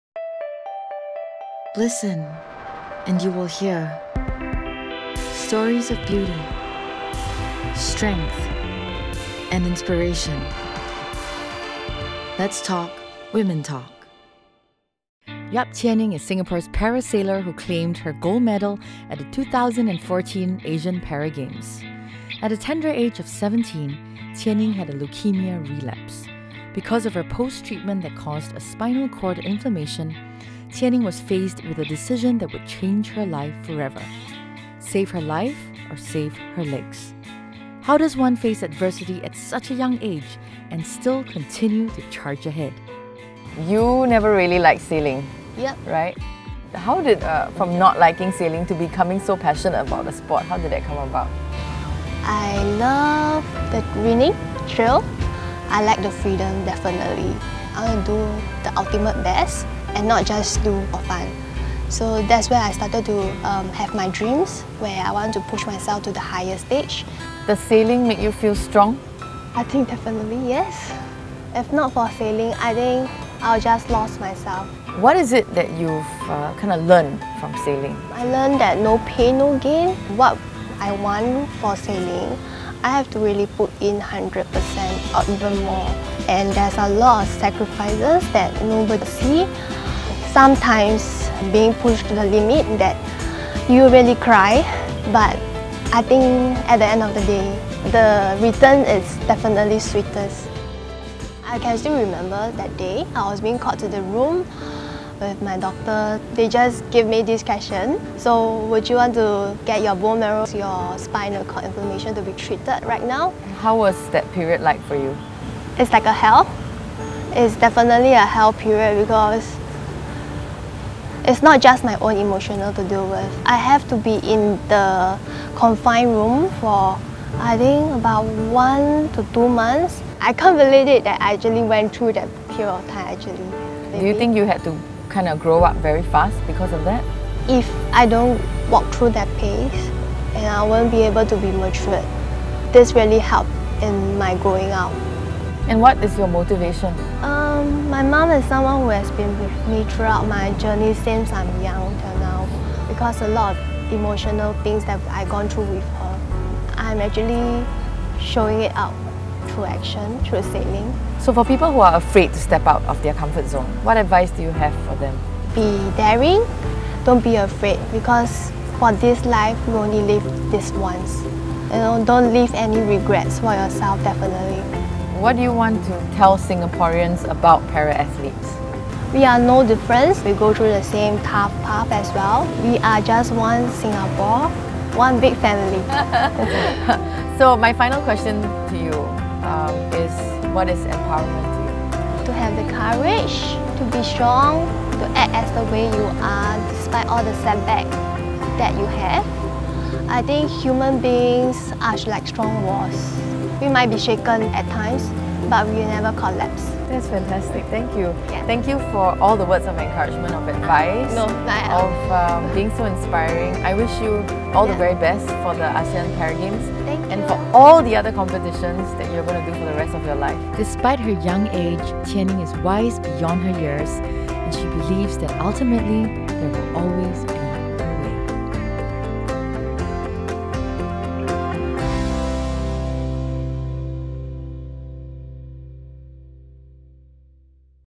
Yap Qian Yin Inspiring ASEAN Para Games sailor from Singapore
In episode 6 of womentalk tv -
"Be daring. Don't be afraid because in this life, you only live once. Don't leave any regrets for yourself." Yap Qian Yin was only 17 when she had a leukemia relapse, and was forced to make a decision to either save her legs, or her life.  Qian Yin overcame all obstacles and now represents Singapore in the ASEAN Para Games. Catch this courageous young leukemia survivor's journey here.